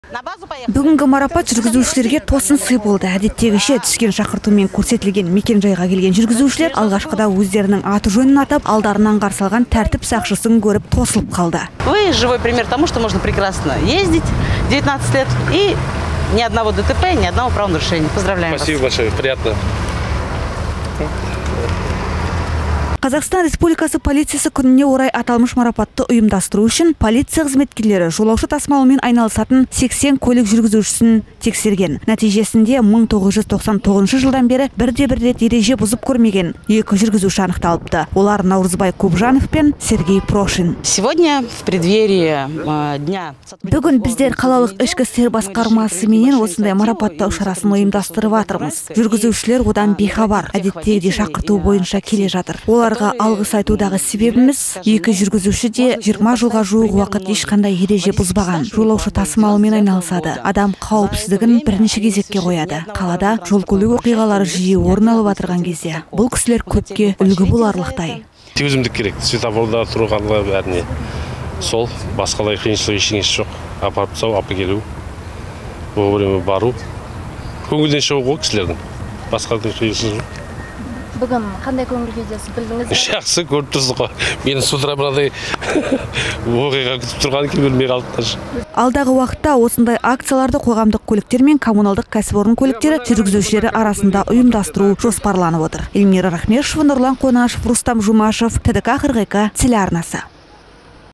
Оралда бұрын-соңды жол ережесін бұзбаған көлік жүргізушілері марапатталды
Олар 20 жылға жуық уақыт өз тәжірибелерінде ешқашан жол ережесін бұзбаған екен. Шақырту бойынша Атоян алаңына жеткен жүргізушілер өздерін жол сақшылары күтіп тұрғанын көріп таңғалып, тосылып қалды. Шара ҚР полициясы күніне орай өткізіліп отыр.

Бүгінгі марапат жүргізушілерге тосын сый болды. Әдеттегіше түскен шақыртумен көрсетілген мекен-жайға келген жүргізушілер алғашқыда өздерінің аты-жөнін атап, алдарынан қарсы алған тәртіп сақшысын көріп, тосылып қалды. Толығырақ: